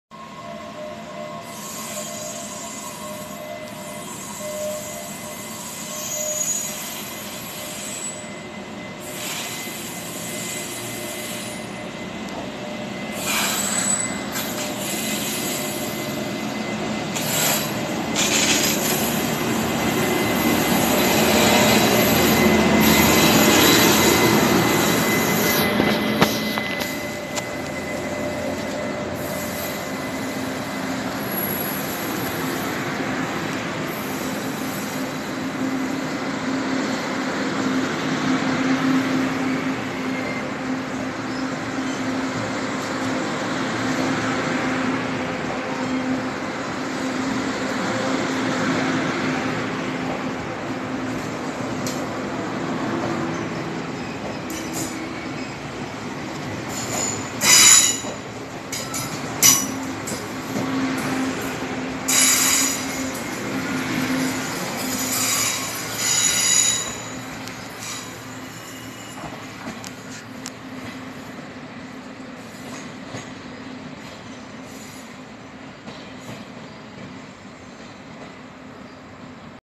2018/10/11 521次莒光號 高雄出站
註:  七堵→潮州(海線)
FPK10500型編組x8

停靠站:  七堵→汐止→松山→臺北→板橋→樹林→鶯歌→桃園→中壢→楊梅→湖口→竹北→新竹→竹南→後龍→通霄→苑裡(交會132/140次自強號)→大甲→清水→沙鹿→彰化→員林→田中→二水→斗六→斗南→大林(待避143&141次自強號)→民雄→嘉義→新營→隆田→善化→永康→臺南→岡山→楠梓→新左營→高雄→鳳山→屏東→潮州